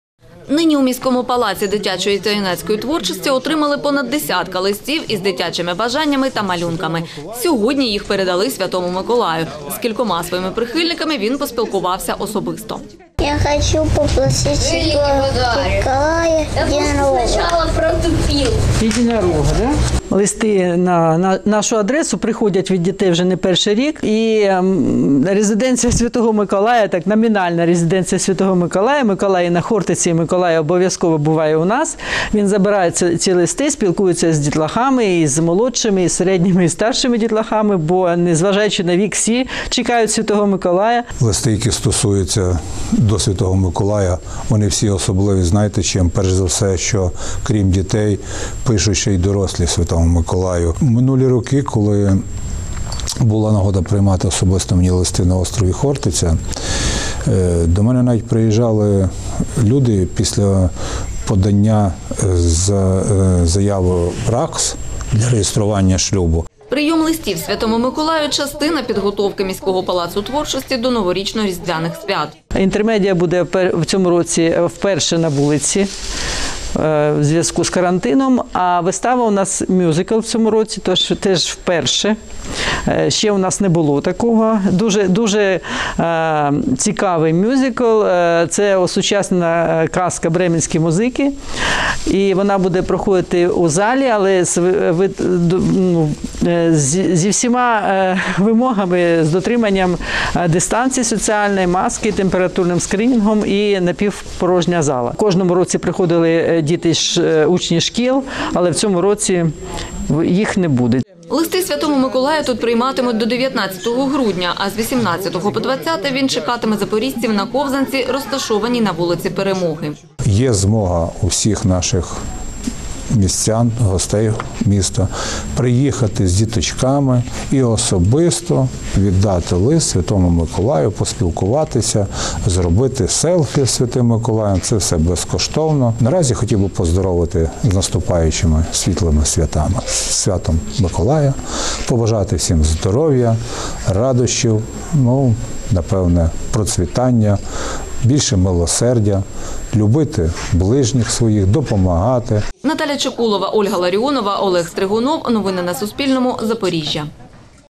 Листи Святому Миколаю | Новини | 17.12.2020
Листи Святому Миколаю пишуть запорізькі діти, готуючись до свята 19 грудня. Зазвичай, приймає він їх на острові Хортиця. Але цьогоріч, через пандемію, курінь Святого Миколая не діє. Листи привозять, або надсилають, тільки до іншої його так званої «резиденції» - міського Палацу дитячої та юнацької творчості.